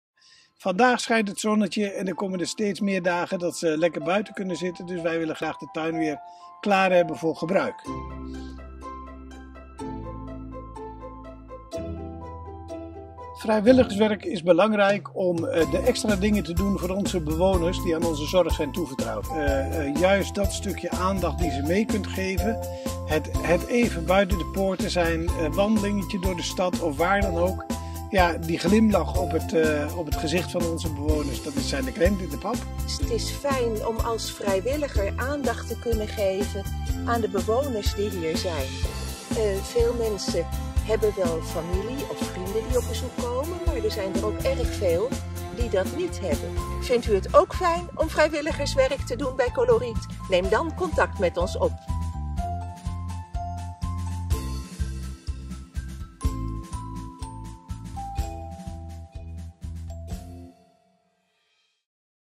Vrijwilligerswerk bij Coloriet Laarhof en Laarstaete in Lelystad
Martin vertelt waarom vrijwilligers zo belangrijk zijn voor ouderen in Lelystad.  Ria is al vrijwilliger en is enthousiast over haar vrijwilligerswerk bij Coloriet Laarhof en Laarstaete.
Wil je ook onze bewoners gezellige momenten bieden? Word dan vrijwilliger. Wil je meer weten? Kijk op